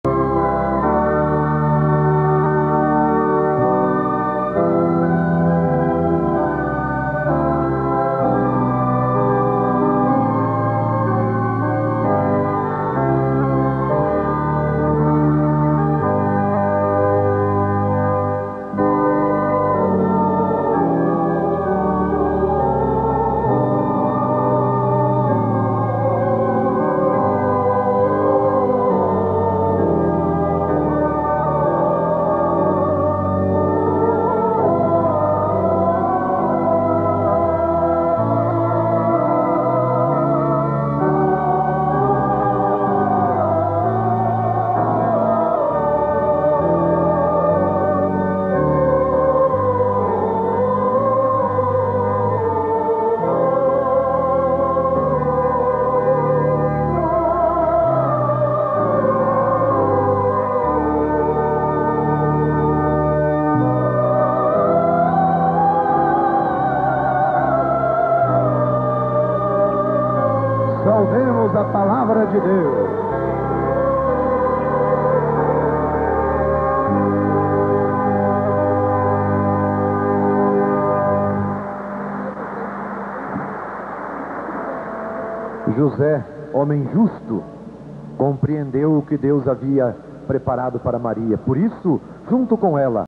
Missa show escandalosa com Dom Fernando Rifan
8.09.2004 - O show missa de Aparicida com a participação de Dom F. Rifan.